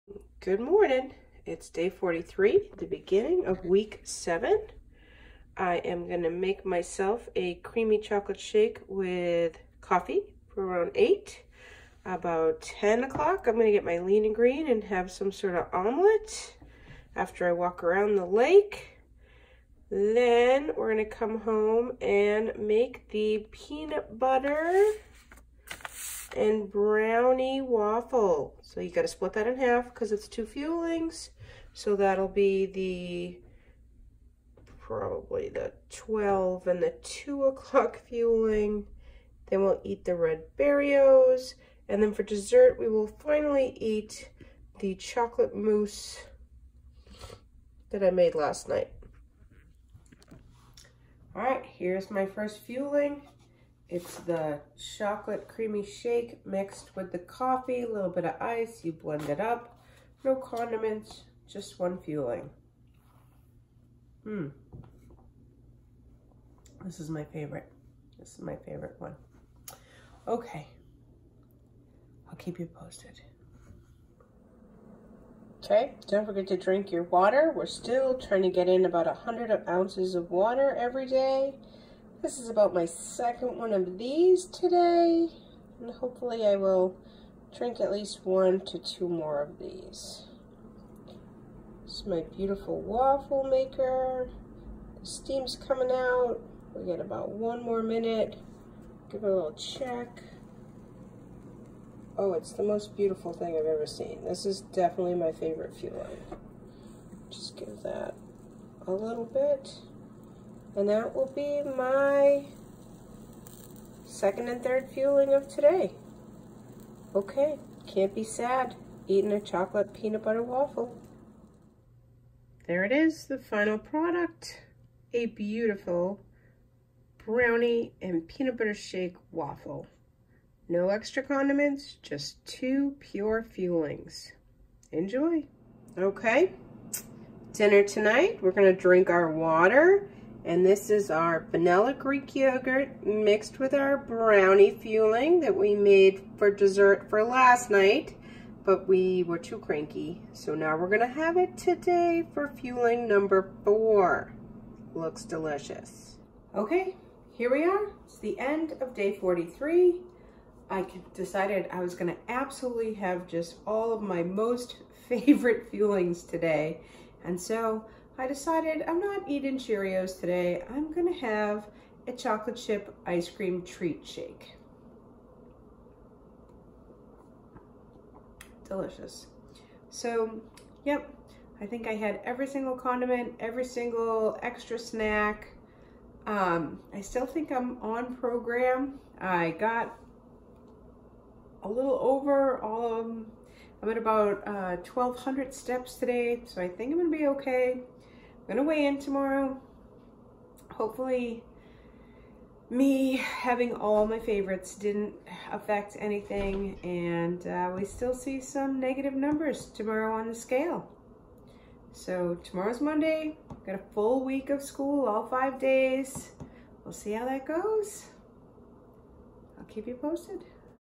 Day 43 All myfavorite Optavia fuelings
100oz of water and all my favorite fuelings
Chocolate shake with my coffee
Brownie and PB waffle = 2 fuelings "can't be sad eating a brownie and PB waffle"
brownie and vanilla yogert treat
Chocolate chip, ice, lt cream cheese frap
All yummy makes me HAPPY!

If you want to know more about getting healthy and eating these fun foods